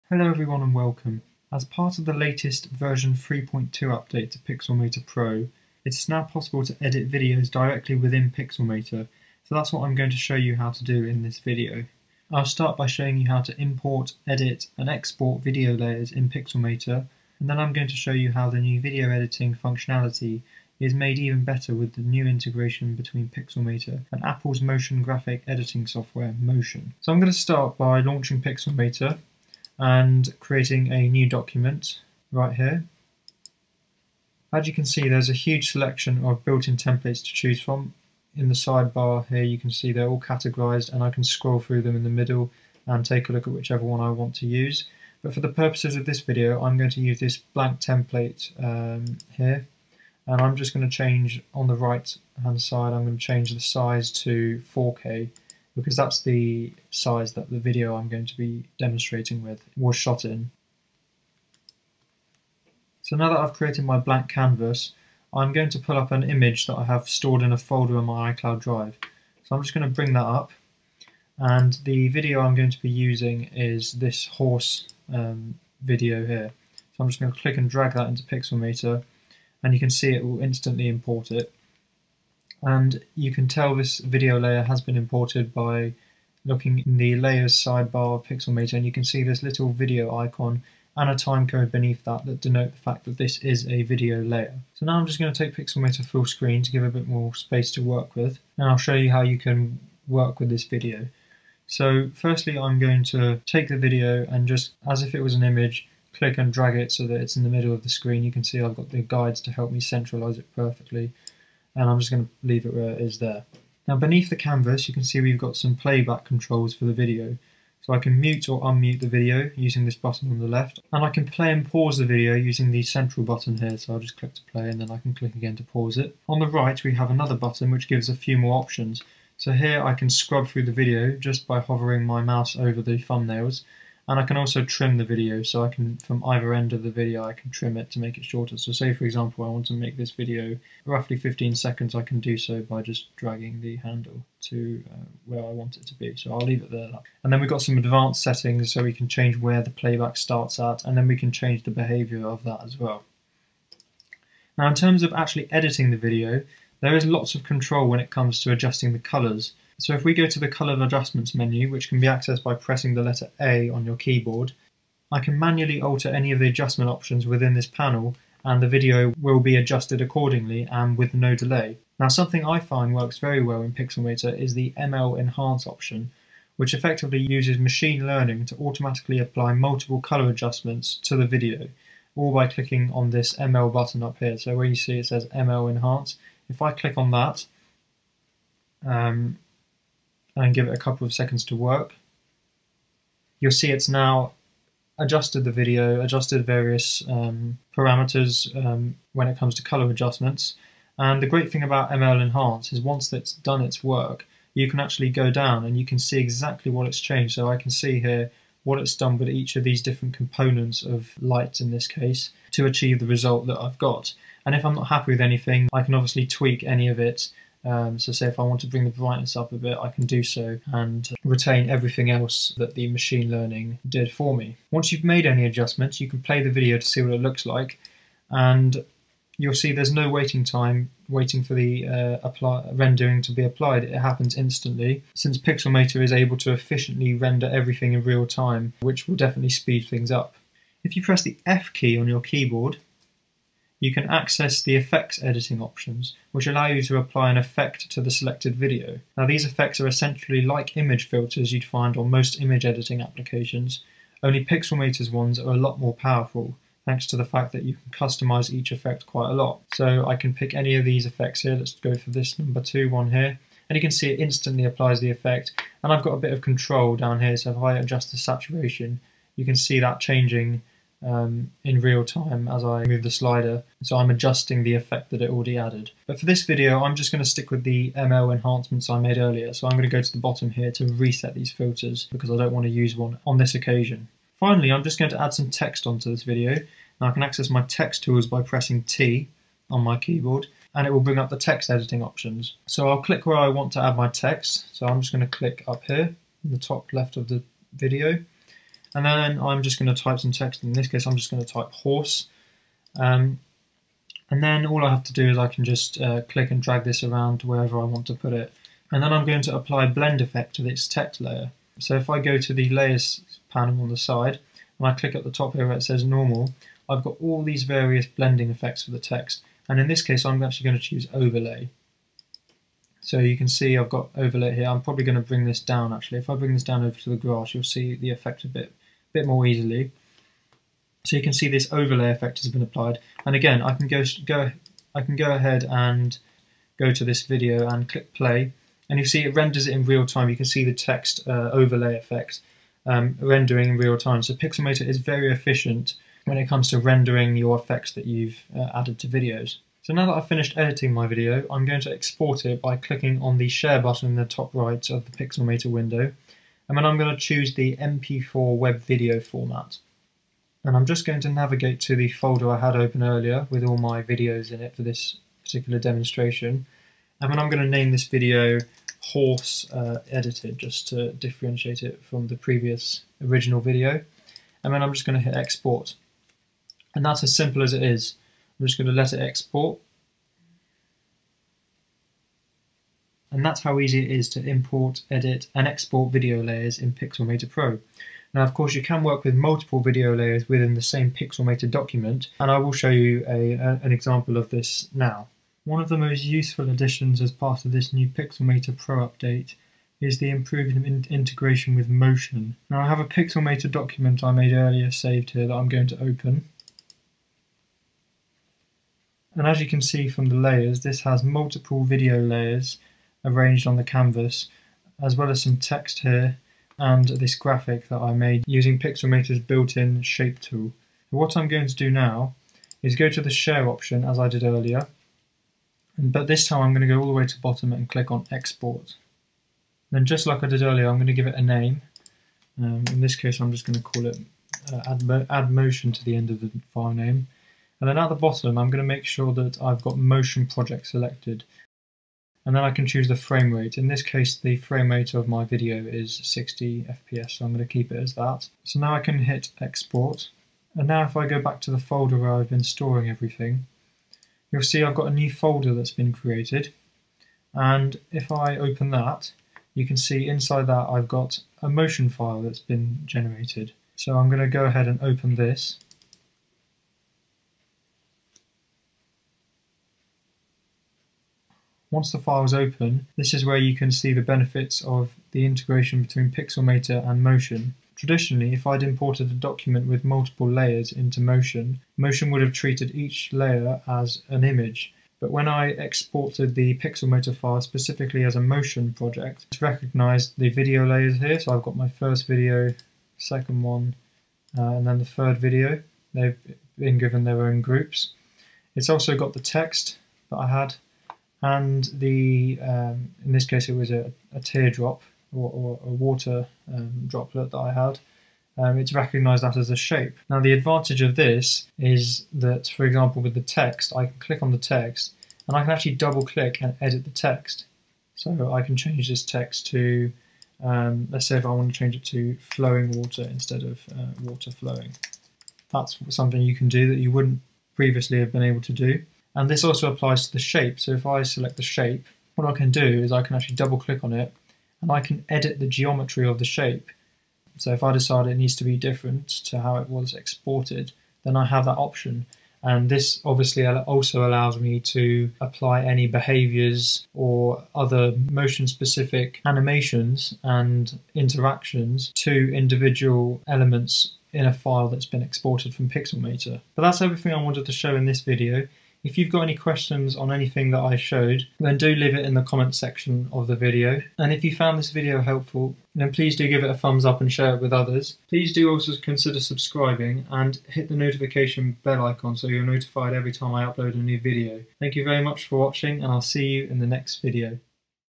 🎥 Edit Video in Pixelmator Pro!
In this video, I show you how you can edit video in Pixelmator Pro! 🎥 I also show you how Pixelmator Pro now integrates well with Apple Motion.

Video Chapters:
00:00 - Intro
00:27 - Importing, Editing and Exporting Video
06:35 - Integration with Apple Motion
09:08 - Outro